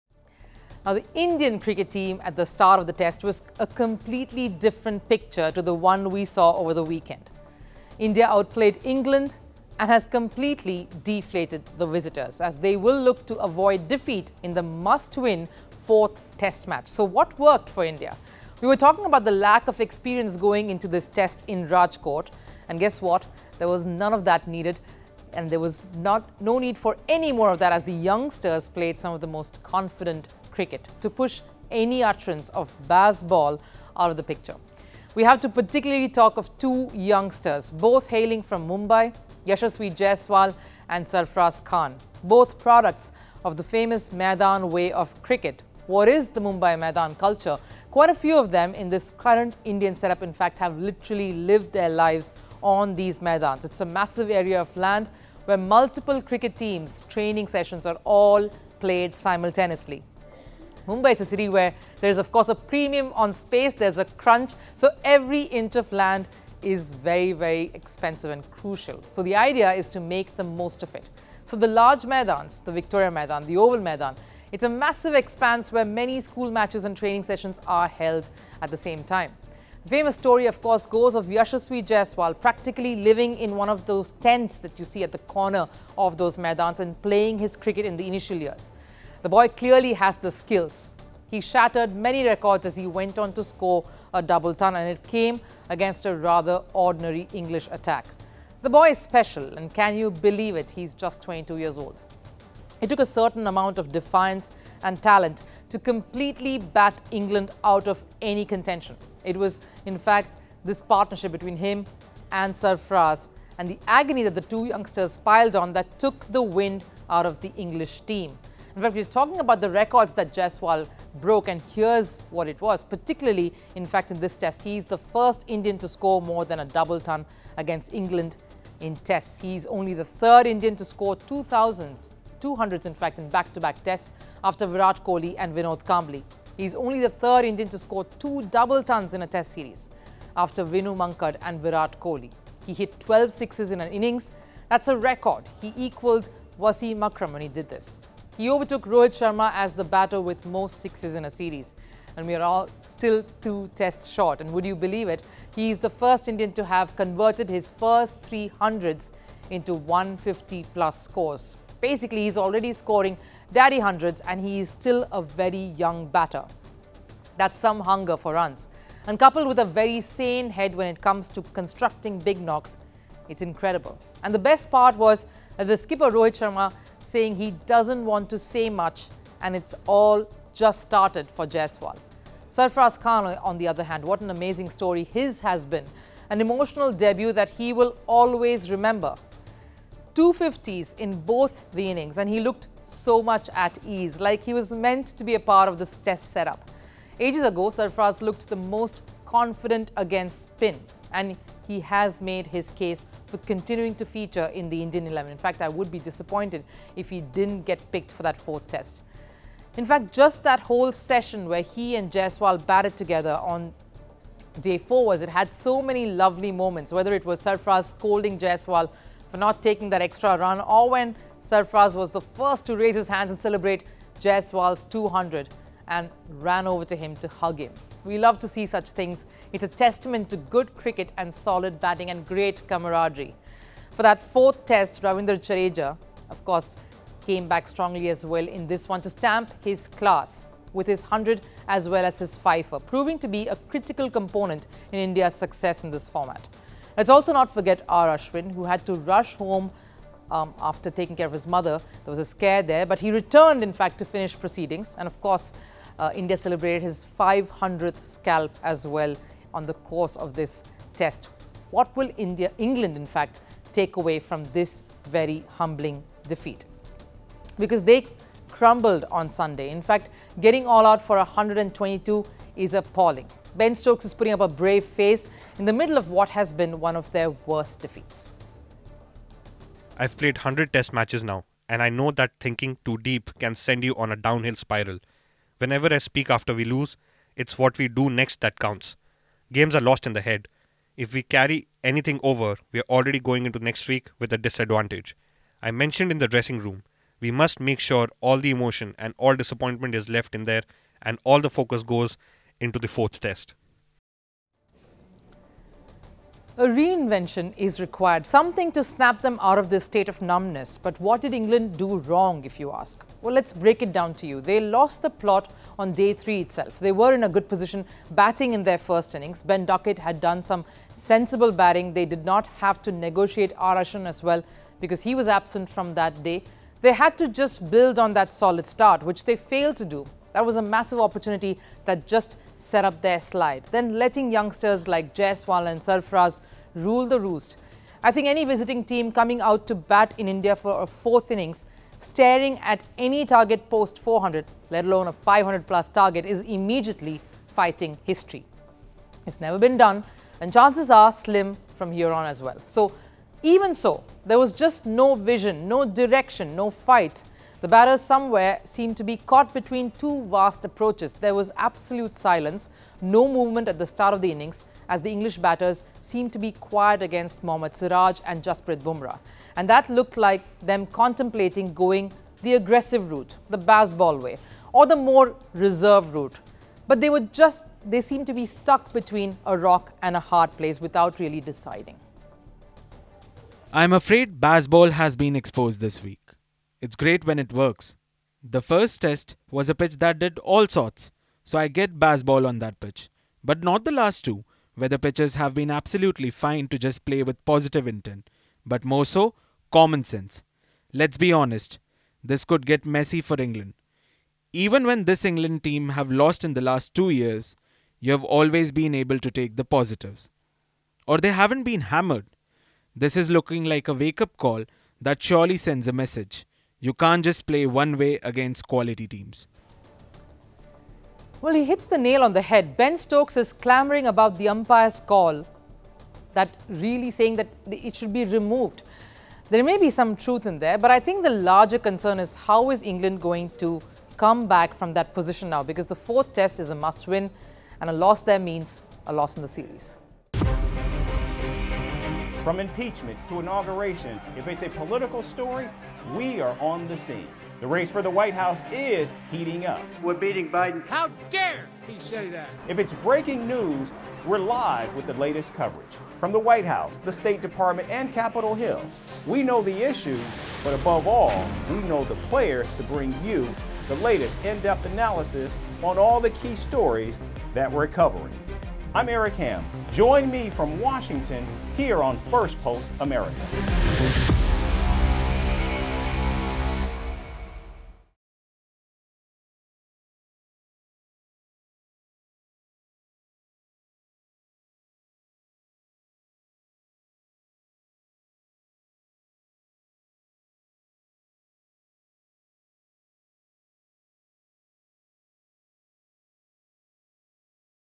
IND V ENG: "Bazball" Under Threat, Jaiswal & Sarfaraz Outbat England |First Sports With Rupha Ramani
England lost the third Test v India by a massive margin of 434 runs. After winning the first Test, England have conceded defeat in consecutive games to go 1-2 down in the series. While Yashasvi Jaiswal, Ravindra Jadeja and Sarfaraz Khan performed for India, England were left clueless at Rajkot. Amidst all the drama, England have faced criticism for their "Bazball" approach. England captain had an interesting take after the loss. Where did they falter and how did India make them pay? Rupha Ramani brings you a post match review.

India | England | Bazball | Yashasvi Jaiswal | Sarfaraz Khan | First Sports | Rupha Ramani | Firstpost | Sports News